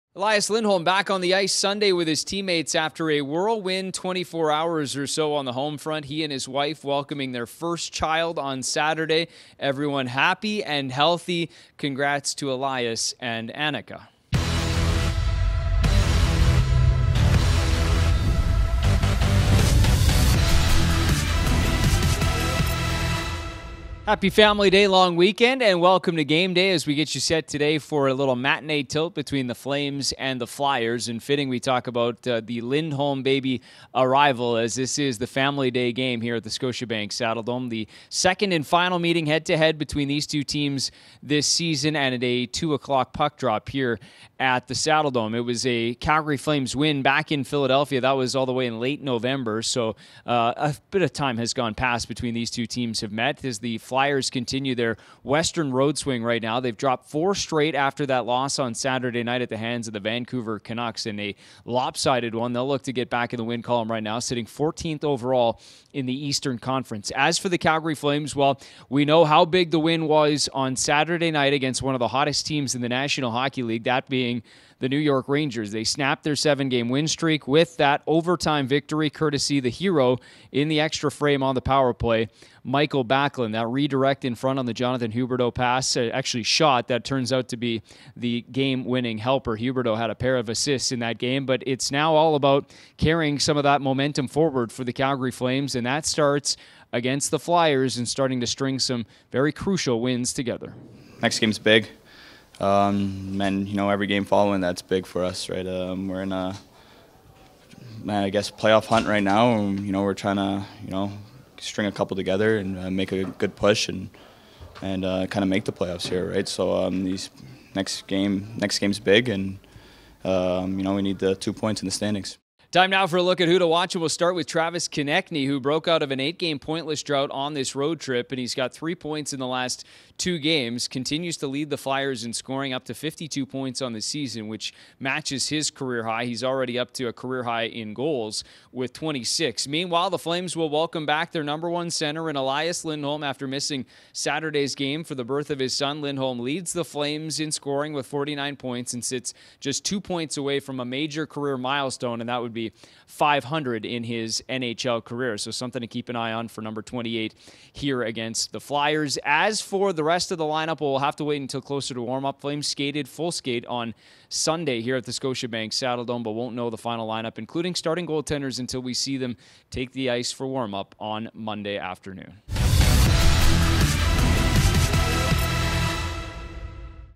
Game Day | Flames vs. Flyers - 20.02.23
Brendan Parker sets up today's Family Day matinee at the 'Dome